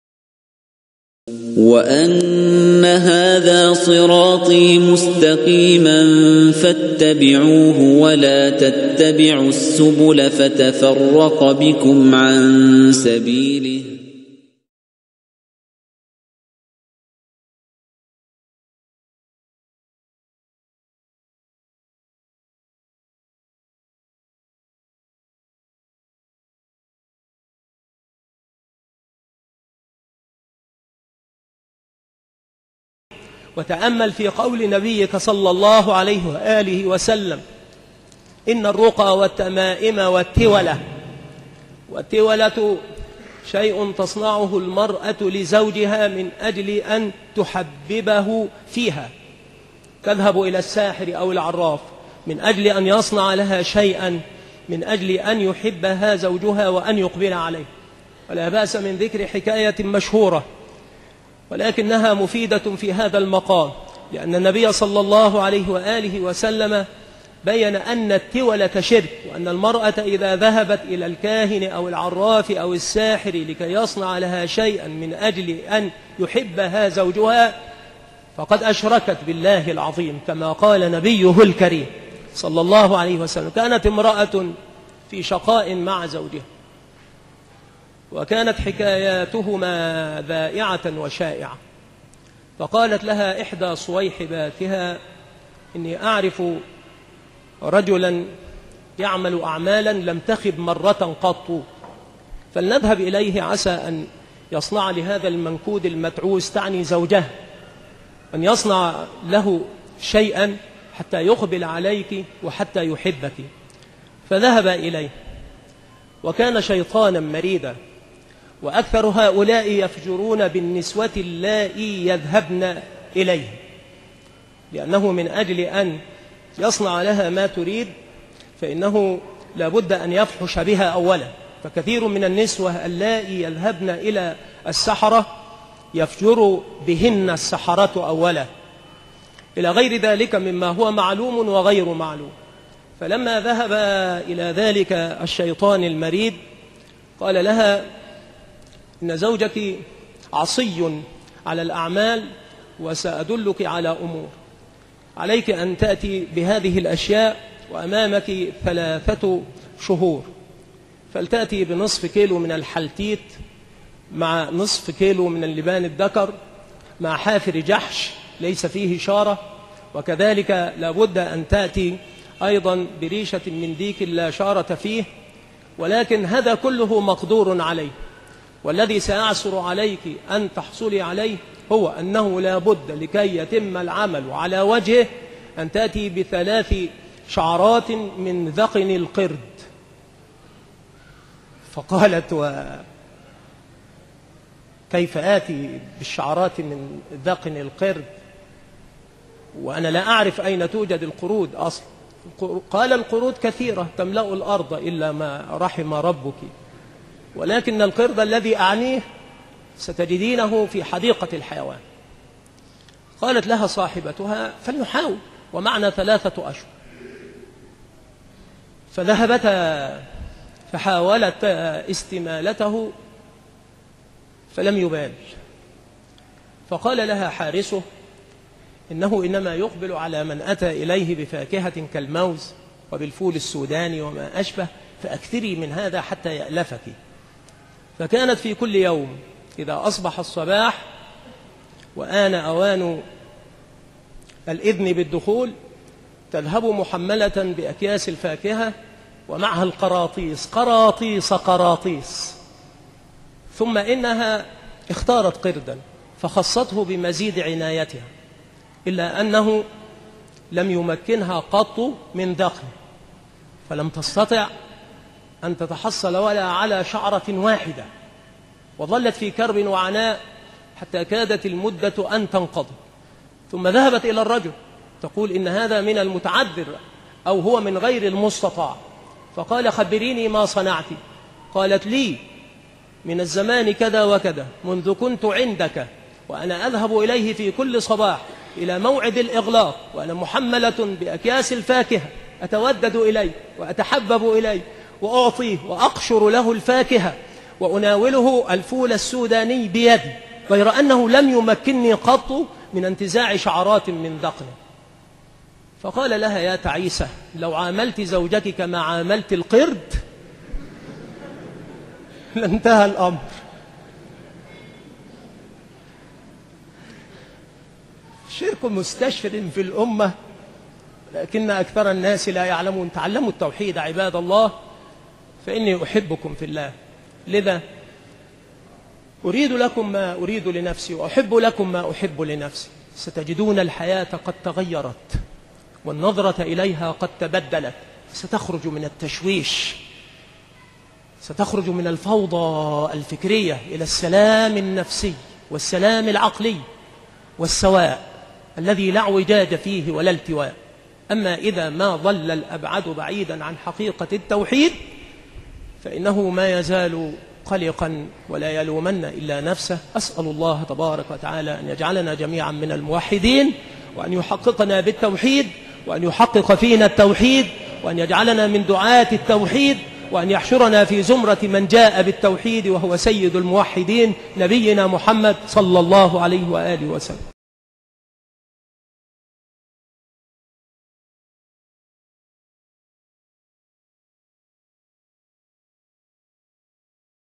l'histoire de la femme et du singe(énorme leçon et conseil)cheikh sa'id raslan
exhortation sur l'unicité et énorme leçon pour celles qui se rendent chez les sorciers ,devins et voyants

Vous pouvez propager les vidéos comme vous le souhaitez mais sans les modifier ni les couper et en mettant la source.
partagez la pour que l'on partage ensemble la récompense.